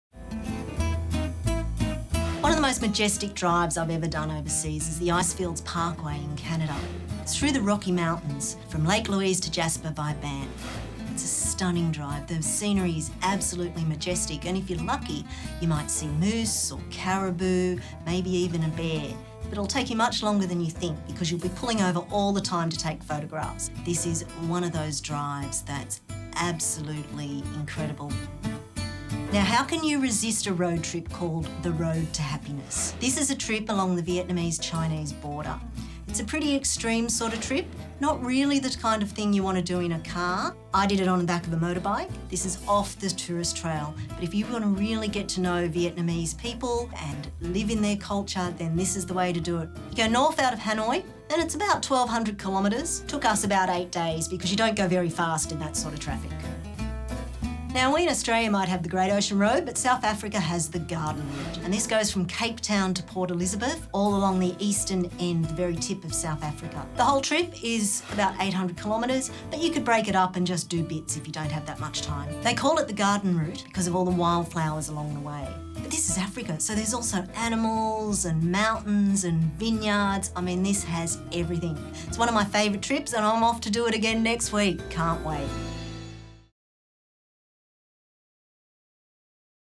Cruise Mode
Lee Atkinson gives us her take on the World's Best Drives!  Are these on your bucket list?